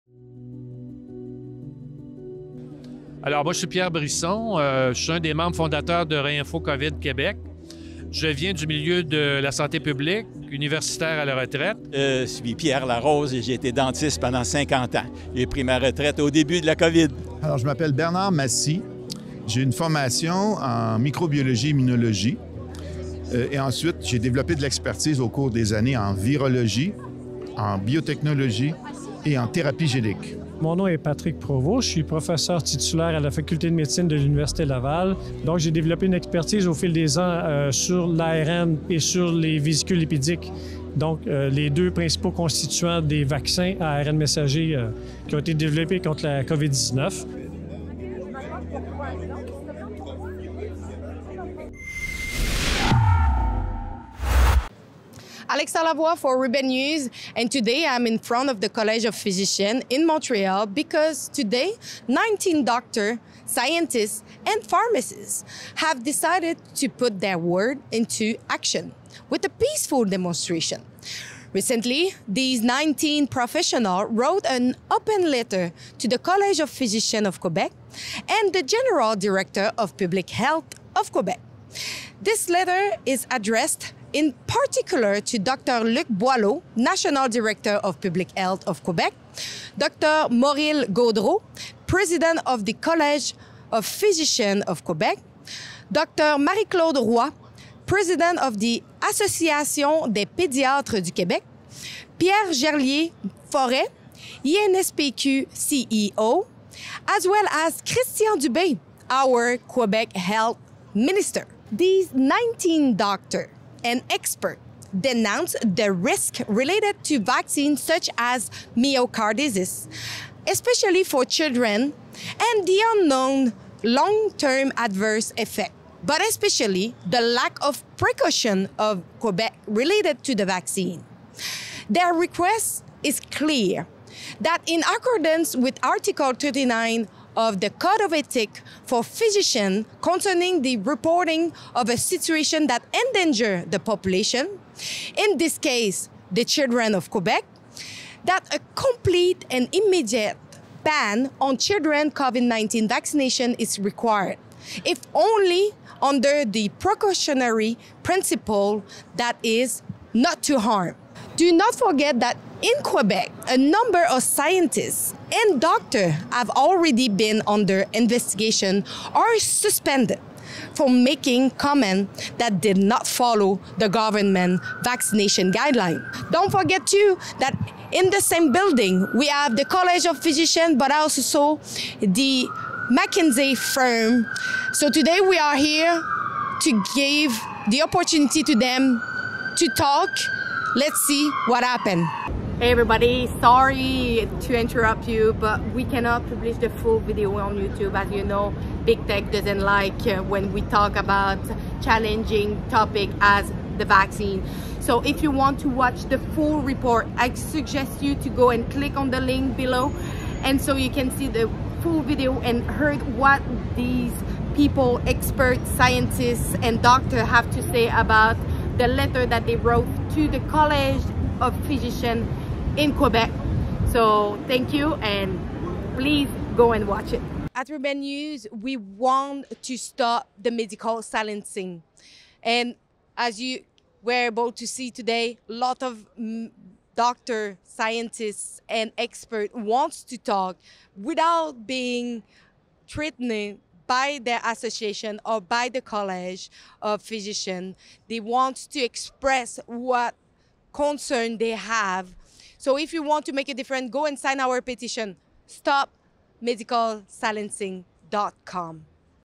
TRAILER: 19 medical professionals are challenging the government's mandatory COVID vaccine for kids
| Many healthcare professionals want to talk, but the fear of retaliation is so great that they choose to keep their mouths shut. Why is it that in 2022, these professionals and experts cannot practice and freely express their field of expertise and their concerns? Quebec is not the only place where several scientists and experts have been muzzled and dragged out into the public arena in order to discredit themselves for speech that varies from the line of government thought.

for more great Rebel content.

Make sure to follow Rebel News' main channel on social media too!